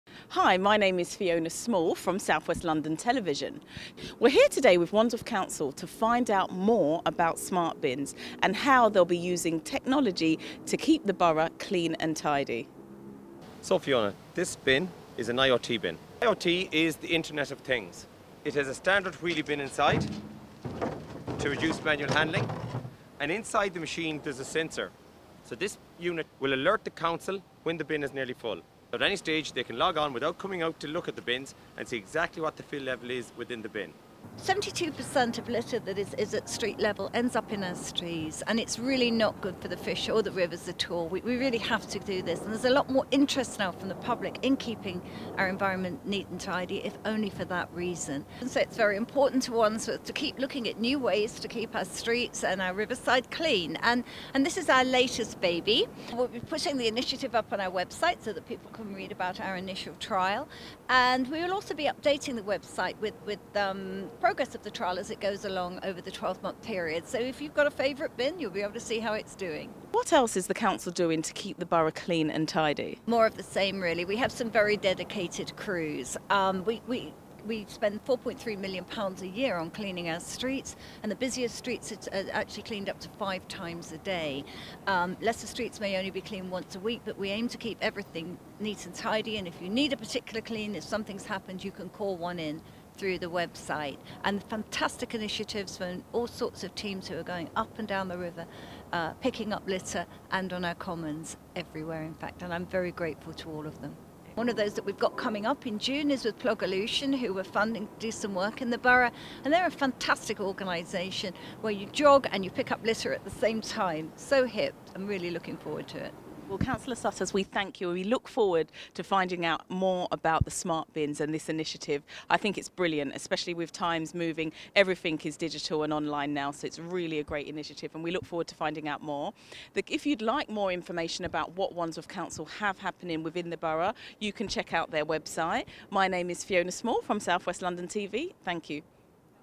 Wandsworth Council launches smart bin trial
Fiona Small follows the story of how people across Wandsworth are trying to keep the borough neat and tidy and protect the environment they live and work in.  This short report highlights an innovative trial of Smart Bins which automatically send an alert when they're getting close to being full.